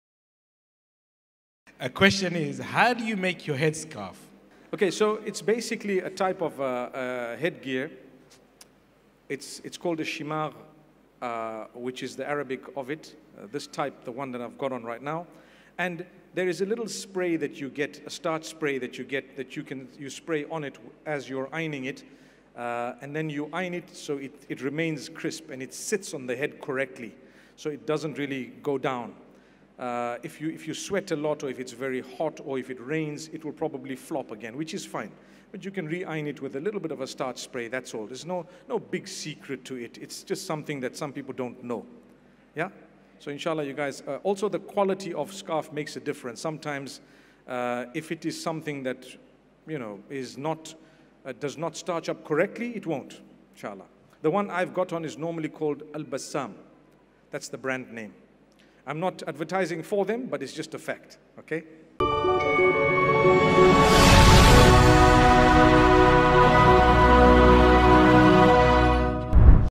How do you make your head scarf ? Talking about himself - Mufti Ismail Menk About his headScarf
Dr. Mufti Ismael Menk is a global Islamic scholar & motivational speaker  prominent Islamic scholar known for his lectures and guidance on various aspects of Islam. In this video, he is talking about himself. he is talking about his scarf.